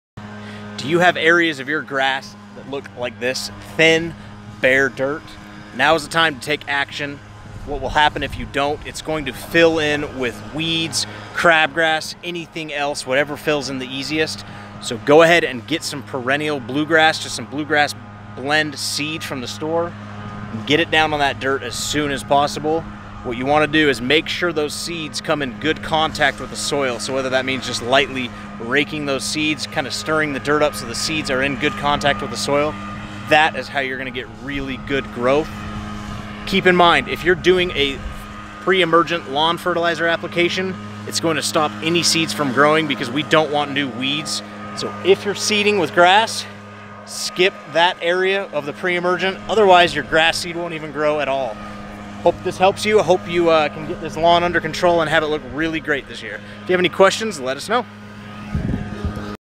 How to get rid of dirt spots in the Lawn
FIX THOSE DIRT SPOTS BEFORE THE WEEDS DO

Now that Spring is here things are going to start growing on any exposed patches of dirt you may have. Seed those patches in your lawn now before the weeds take it over.